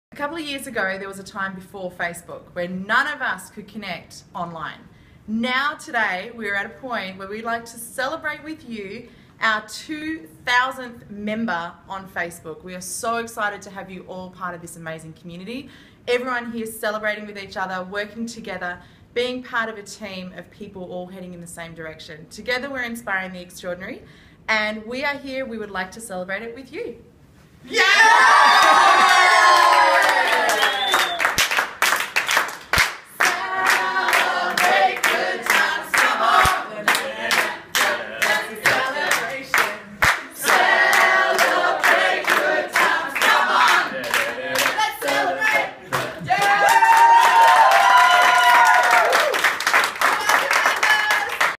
2000 members on Facebook!
Facebook has grown so much over the last 2 years and our 'coach only' group now has 2000 active members sharing, connecting and growing together all over the world! We are weird so we decided to sing :)